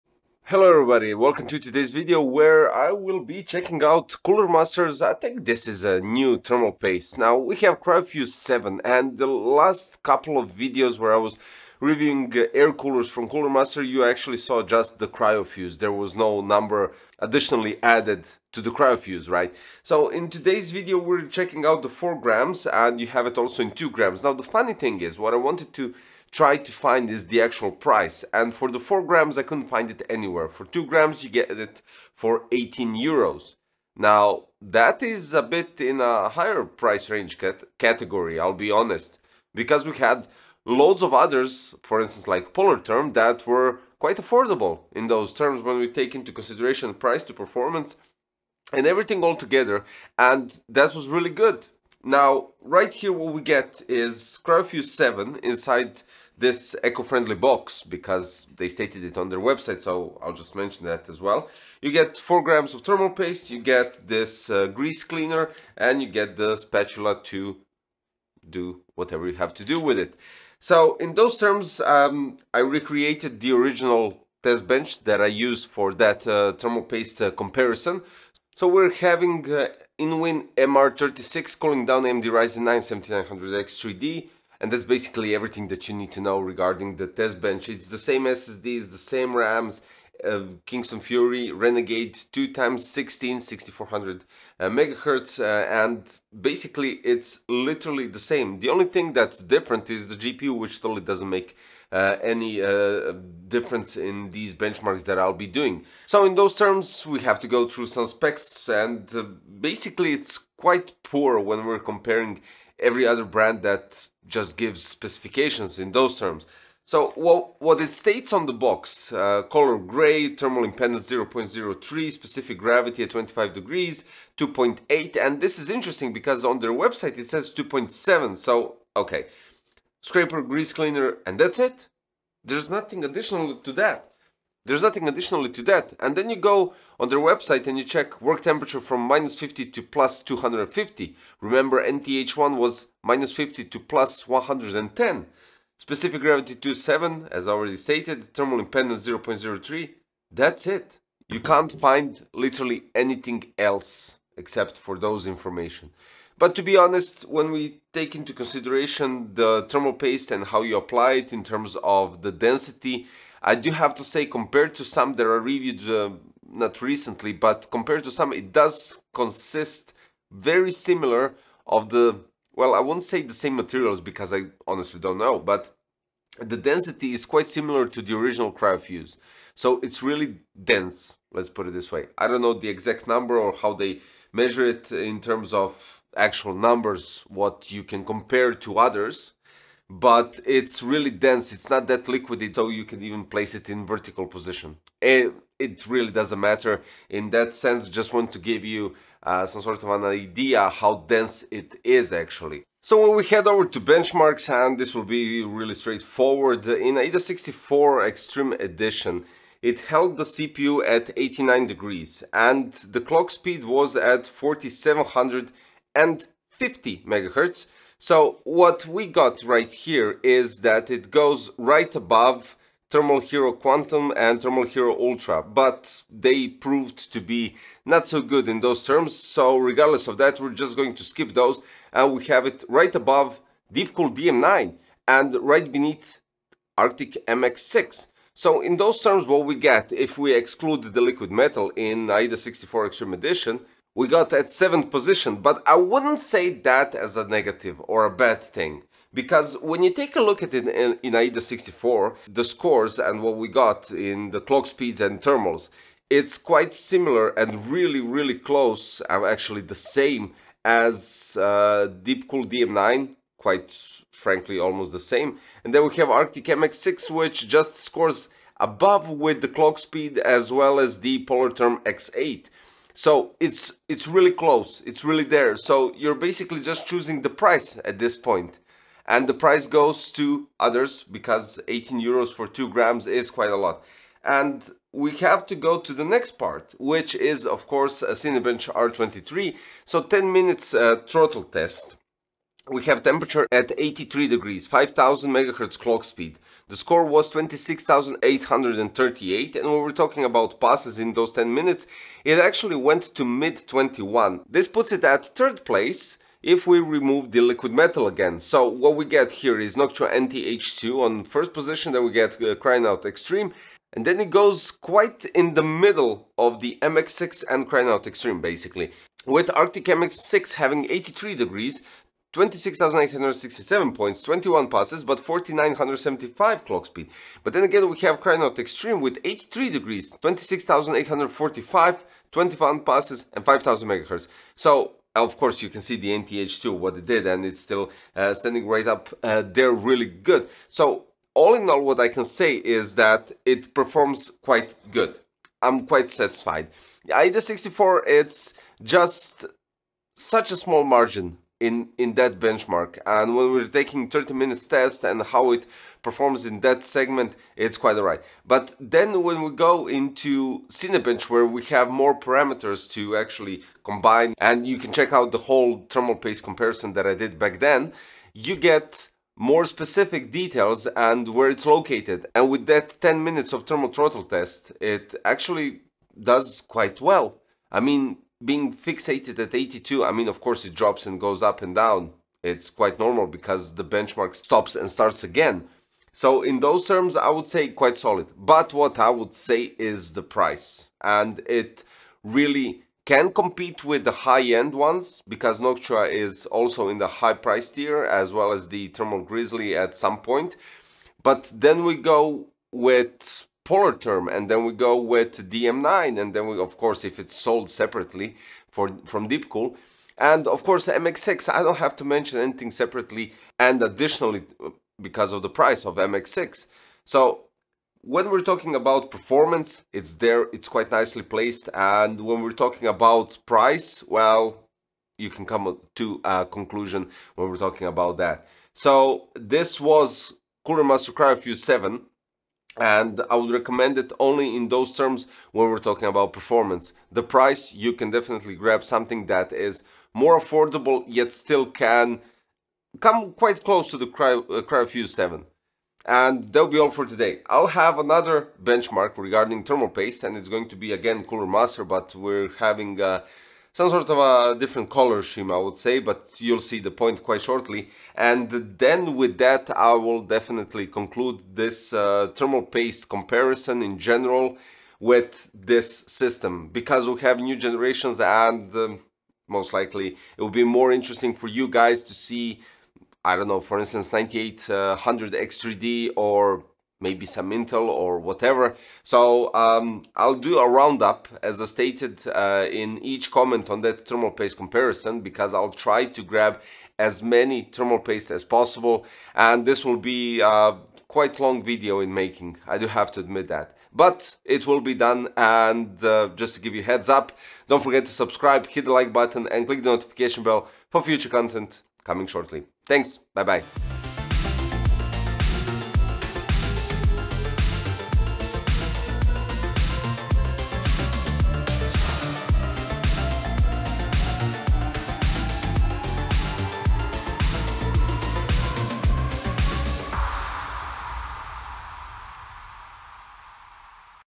Great Performance With A Catch - Cooler Master CryoFuze 7
High density, great performance and even stacks up quite nicely compared to other thermal pastes. This is Cooler Master CryoFuze 7 that comes in 2 and 4g package.

My recording gear:
►Lighting System: NANLITE FS-150
►Softboxes: NANLITE SB-PR-90-Q
►RGB Tubes: NANLITE PavoTube II 6C